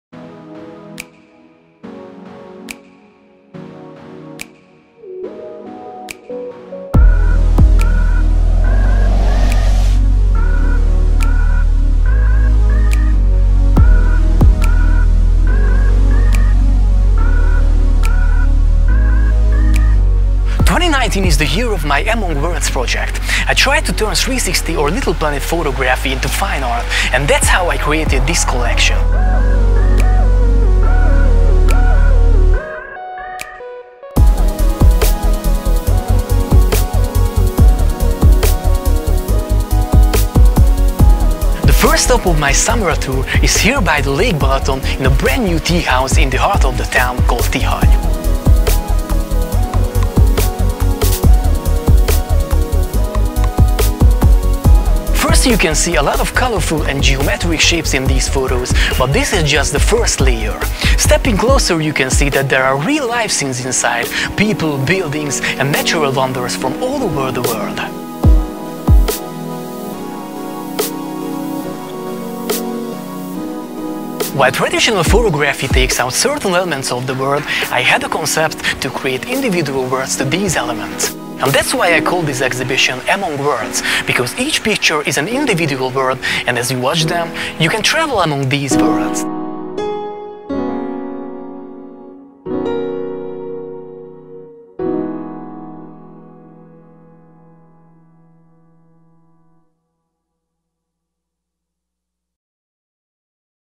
Among Worlds: 360 photo exhibition opening by the Lake Balaton | 360 photography meets fine art
/AMONG_WORLDS: The exhibition where 360 photography meets fine art. The first 360 photography exhibition in Hungary moves to the Lake Balaton, into the heart of Tihany.

Location:
Hungary, Tihany, Batthyány utca 18.
Örökké Tavasz - Kávé és teaház (Legendás Ház)

The exhibition is open every day from 8 am to 6 pm.

Photography shows us the essence of the world in a subjective way, letting us to watch through the photographer’s eyes at a particular location in a particular time. In the meantime, we have the illusion that the subject of the photograph has an independent world, like it could exist in itself, without the photographer - but the photographer is always there with camera in hands.

I decided to create my own little planets, where nothing remains hidden from the viewers' eyes. My photos have their own worlds, telling stories without any photographers hiding in the background. You only see the whole individual universes.

That’s how the planet of the neon world of Corvin district was born among further colorful locations from all over Europe. And that’s how I made my abstract images, showing different moods through my unusual landscapes.

This is what happens when we observe every single part of the life as whole universes. And although we can find the beauty in each world, the most exciting experience is to travel among them.